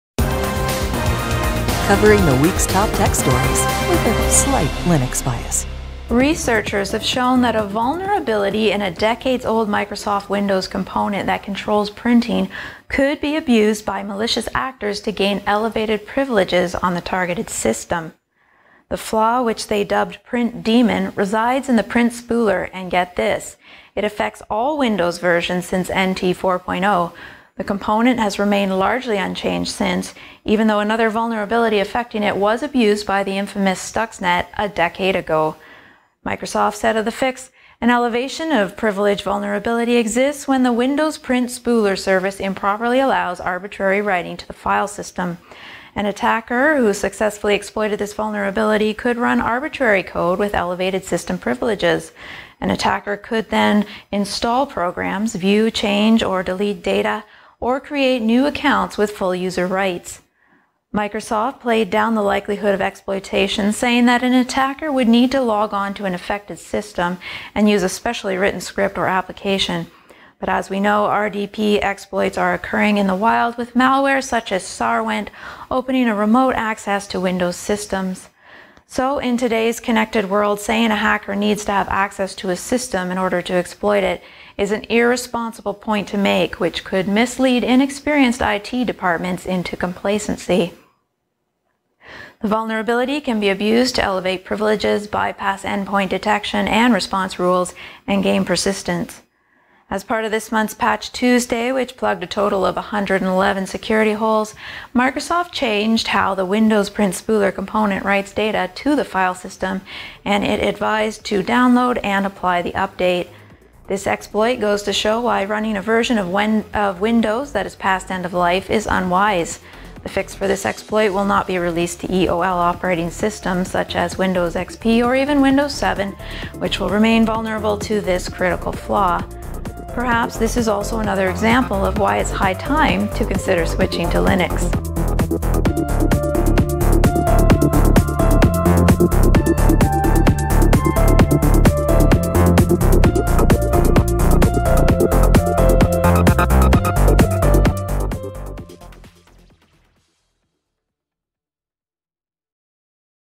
Microsoft has fixed a critical vulnerability affecting ALL Windows versions since 1996
Researchers have shown that a vulnerability in a decades-old Microsoft Windows component that controls printing could be abused by malicious actors to gain elevated privileges on the targeted system, and it's been sitting in plain sight in every version of Windows since NT 4.0.